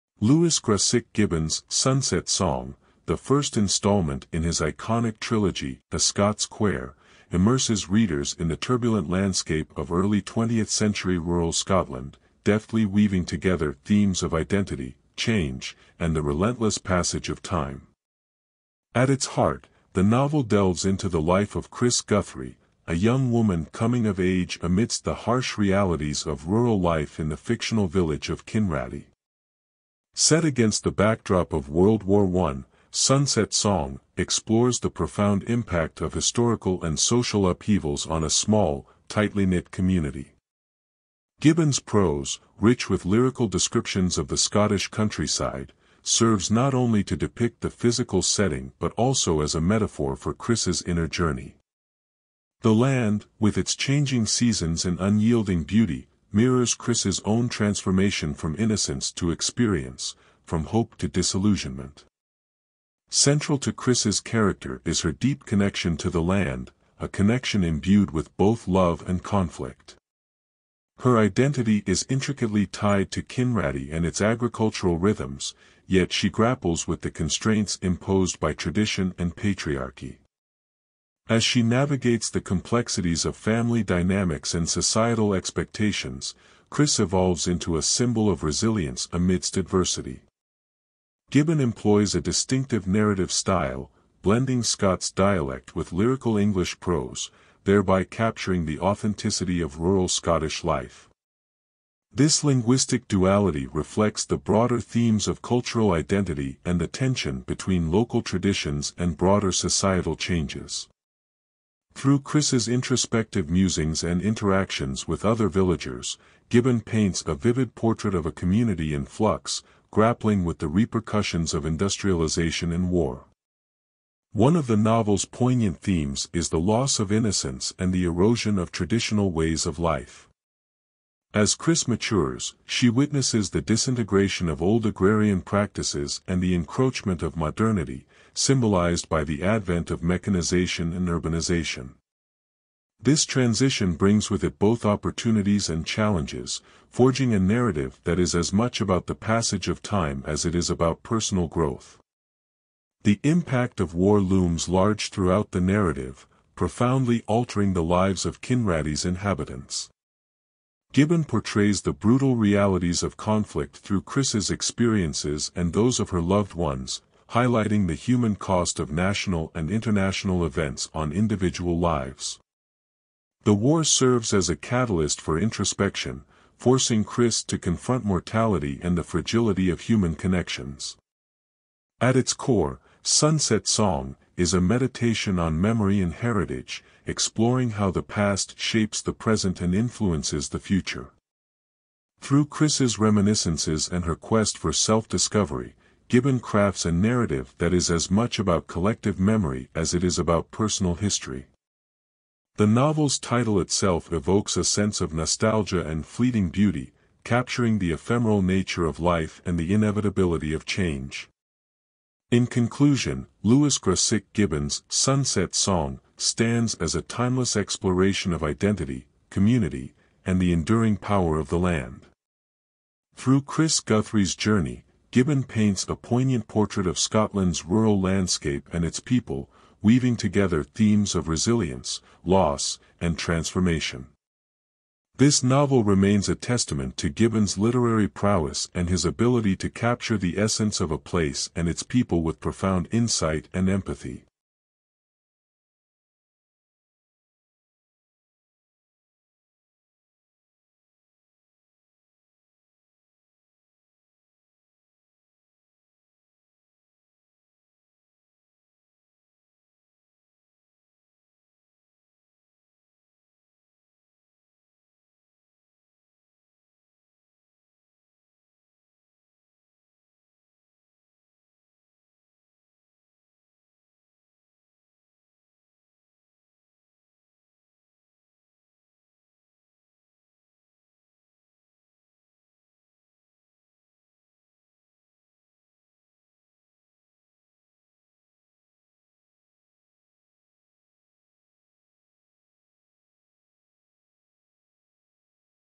"Sunset Song (A Scots Quair, #1)" By Lewis Grassic Gibbon
Lewis Grassic Gibbon's "Sunset Song," the first installment in his iconic trilogy "A Scots Quair," immerses readers in the turbulent landscape of early 20th-century rural Scotland, deftly weaving together themes of identity, change, and the relentless passage of time. At its heart, the novel delves into the life of Chris Guthrie, a young woman coming of age amidst the harsh realities of rural life in the fictional village of Kinraddie.Set against the backdrop of World War I, "Sunset Song" explores the profound impact of historical and social upheavals on a small, tightly-knit community. Gibbon's prose, rich with lyrical descriptions of the Scottish countryside, serves not only to depict the physical setting but also as a metaphor for Chris's inner journey. The land, with its changing seasons and unyielding beauty, mirrors Chris's own transformation from innocence to experience, from hope to disillusionment.Central to Chris's character is her deep connection to the land, a connection imbued with both love and conflict. Her identity is intricately tied to Kinraddie and its agricultural rhythms, yet she grapples with the constraints imposed by tradition and patriarchy. As she navigates the complexities of family dynamics and societal expectations, Chris evolves into a symbol of resilience amidst adversity.Gibbon employs a distinctive narrative style, blending Scots dialect with lyrical English prose, thereby capturing the authenticity of rural Scottish life. This linguistic duality reflects the broader themes of cultural identity and the tension between local traditions and broader societal changes. Through Chris's introspective musings and interactions with other villagers, Gibbon paints a vivid portrait of a community in flux, grappling with the repercussions of industrialization and war.One of the novel's poignant themes is the loss of innocence and the erosion of traditional ways of life. As Chris matures, she witnesses the disintegration of old agrarian practices and the encroachment of modernity, symbolized by the advent of mechanization and urbanization. This transition brings with it both opportunities and challenges, forging a narrative that is as much about the passage of time as it is about personal growth.The impact of war looms large throughout the narrative, profoundly altering the lives of Kinraddie's inhabitants. Gibbon portrays the brutal realities of conflict through Chris's experiences and those of her loved ones, highlighting the human cost of national and international events on individual lives. The war serves as a catalyst for introspection, forcing Chris to confront mortality and the fragility of human connections.At its core, "Sunset Song" is a meditation on memory and heritage, exploring how the past shapes the present and influences the future. Through Chris's reminiscences and her quest for self-discovery, Gibbon crafts a narrative that is as much about collective memory as it is about personal history. The novel's title itself evokes a sense of nostalgia and fleeting beauty, capturing the ephemeral nature of life and the inevitability of change.In conclusion, Lewis Grassic Gibbon's "Sunset Song" stands as a timeless exploration of identity, community, and the enduring power of the land. Through Chris Guthrie's journey, Gibbon paints a poignant portrait of Scotland's rural landscape and its people, weaving together themes of resilience, loss, and transformation. This novel remains a testament to Gibbon's literary prowess and his ability to capture the essence of a place and its people with profound insight and empathy.